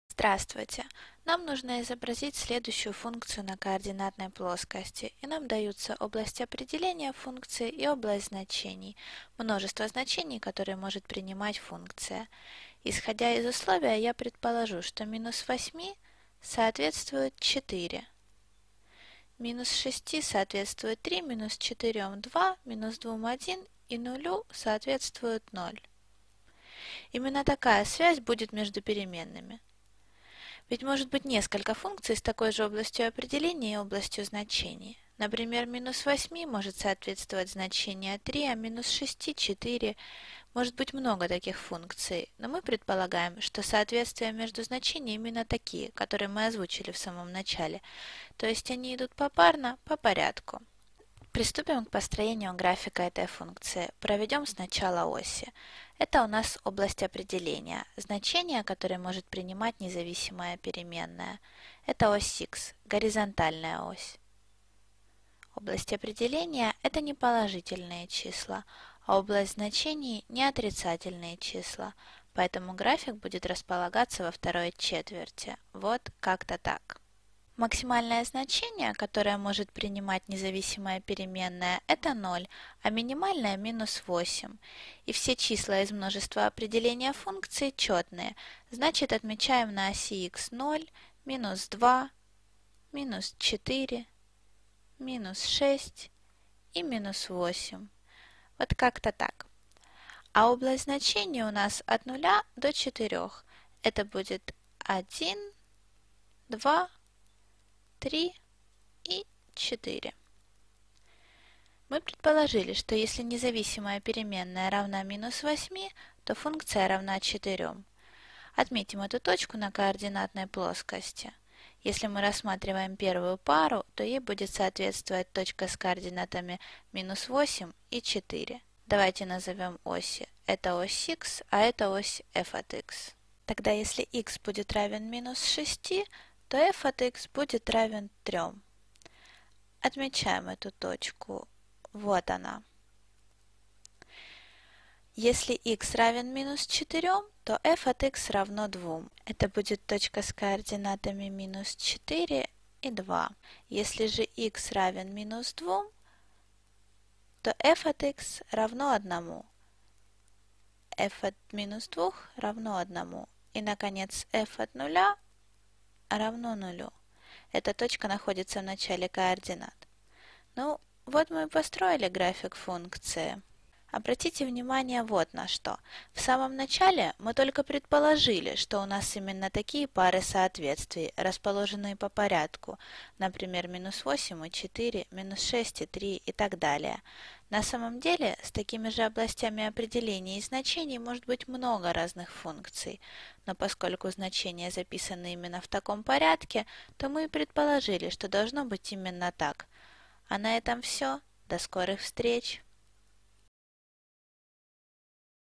Области определения и значений 2
В данном видео показано, как изобразить функцию на координатной плоскости.

Это видео - русская версия видео «Domain and Range 2» Академии Хана Перевод и дублирование выполнены командой проектов «Edukit» и «Study Planner»

This video is a Russian dubbed version of the Khan Academy video "Domain and Range 2" The translation and sampling are made by the "Edukit" and "Study Planner" team